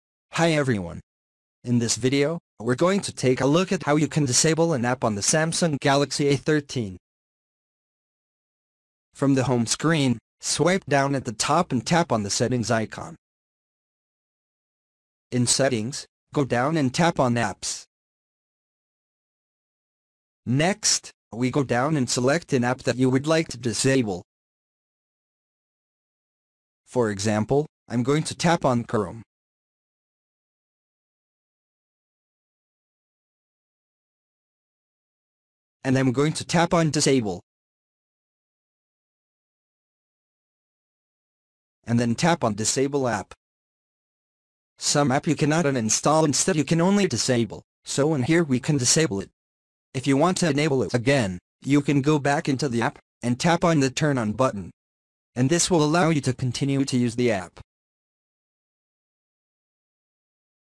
Samsung Galaxy A13: How to DISABLE AN APP
Learn how you can DISABLE AN APP on the Samsung Galaxy A13.